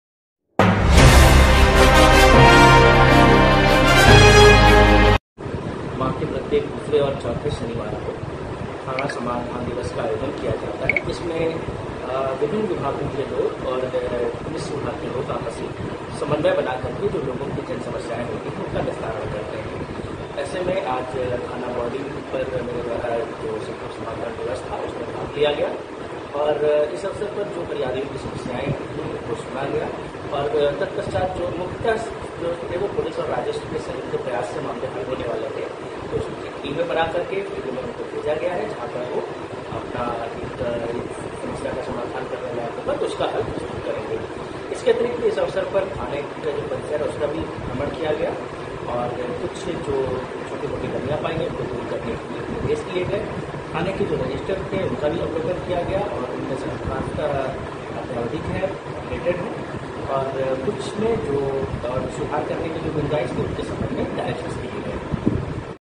Bahraich: SP प्रशान्त वर्मा ने जिले के बौंडी थाने का आज किया औचक निरीक्षण,
Bahraich: SP प्रशान्त वर्मा ने जिले के बौंडी थाने का आज किया औचक निरीक्षण, वहीं थाना समाधान दिवस में आए फरियादियों की सुनी फरियाद
अभिलेखों के रख रखाव,महिला हेल्प डेस्क, कंप्यूटर कक्ष, मेस व परिसर की साफ-सफाई का लिया जायजा,संबंधित को दिए आवश्यक दिशा- निर्देश